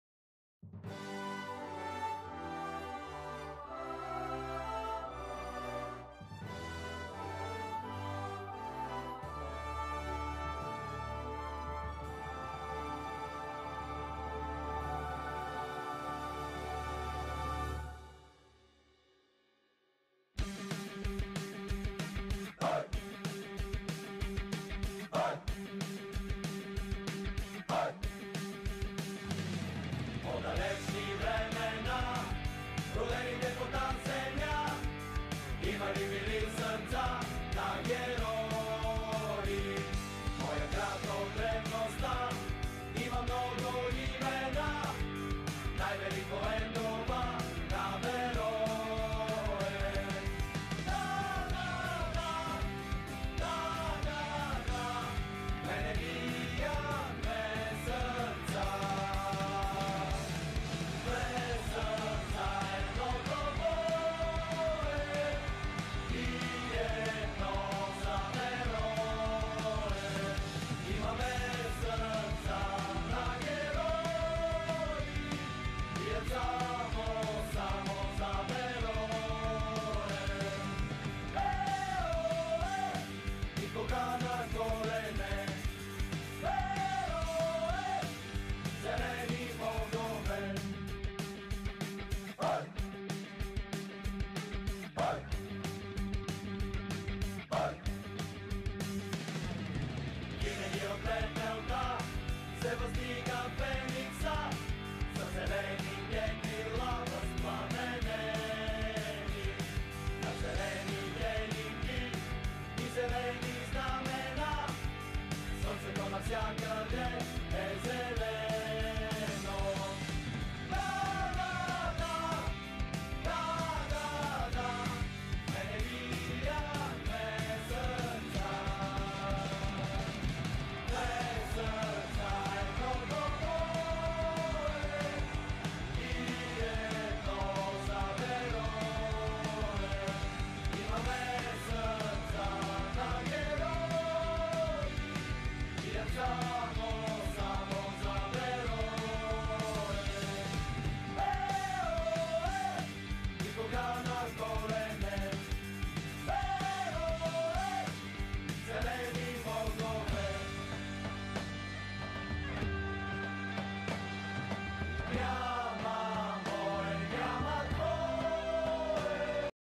"Берое" - "Марица" 3:1 (репортаж)
Репортаж с най-интересните моменти от контролата между "Берое" и "Марица"